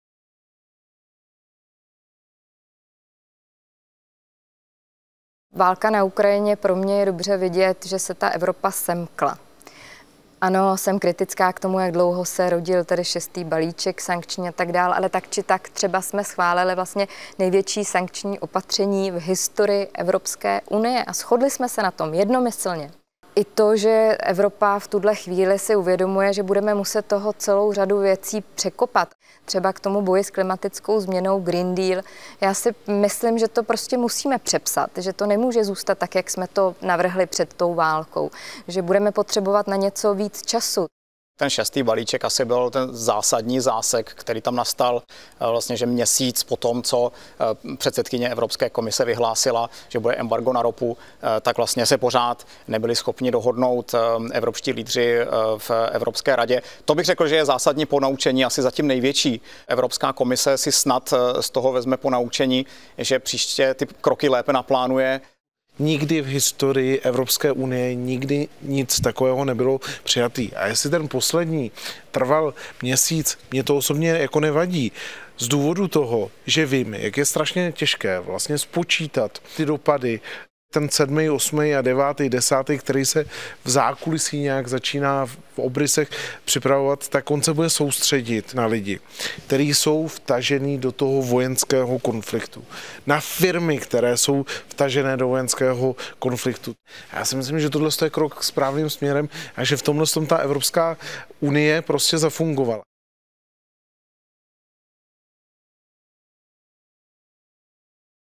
Debata s českými europoslanci: sestřih III
Jedním z témat, které probírala ČTK Protext s českými europoslanci po skončení druhého dne plenárního zasedání ve Štrasburku dne 7. června, byla situace na Ukrajině. V textu shrnujeme názory, jež zazněly, stejně jako dosavadní opatření ze strany EU na pomoc napadené zemi.